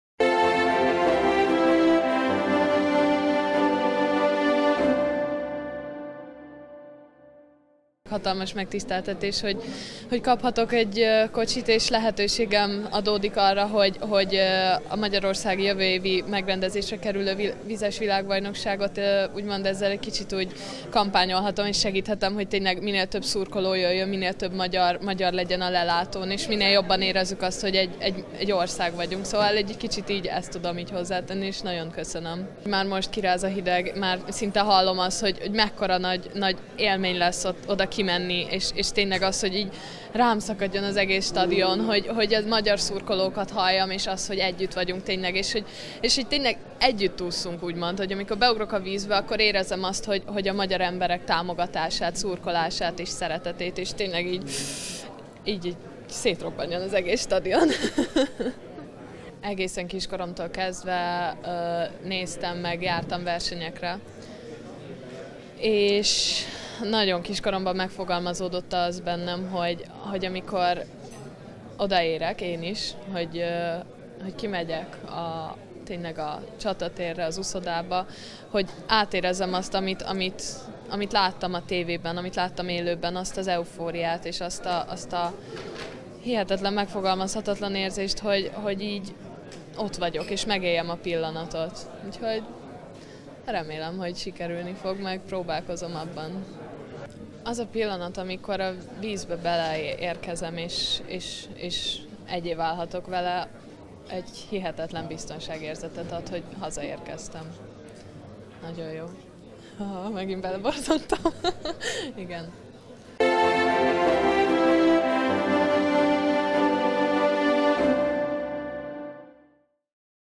Szilágyi Liliána: „Szerintem jól elleszünk majd ketten”
A 17. FINA Világbajnokságra készülő magyar úszóválogatott tagjai újabb segítséget kaptak, ezúttal egy gépkocsival gazdagodtak a vb-felkészülés jegyében.